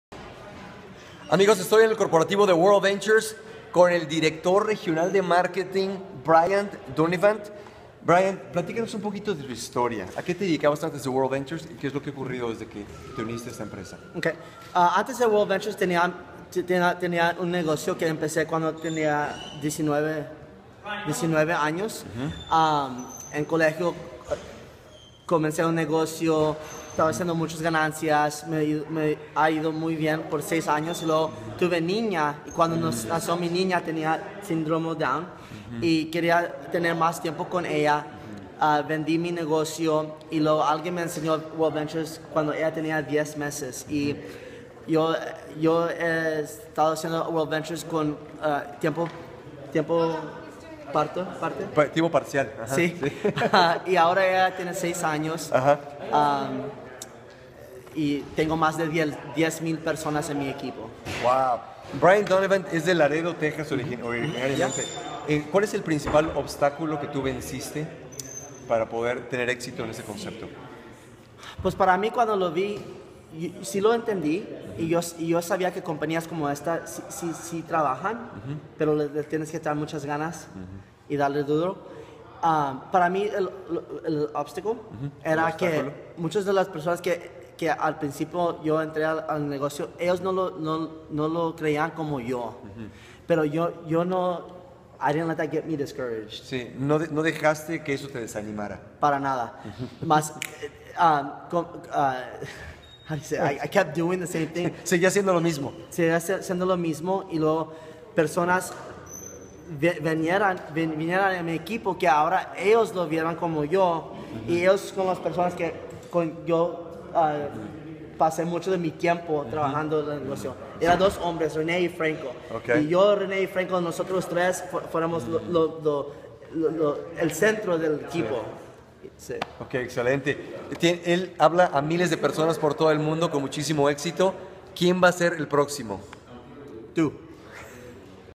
Testimonio: dueño de negocio
Brian Dunivant era dueño de negocio. Ahora es RMD con worldventures y tiene más de 10,000 miembros en su equipo.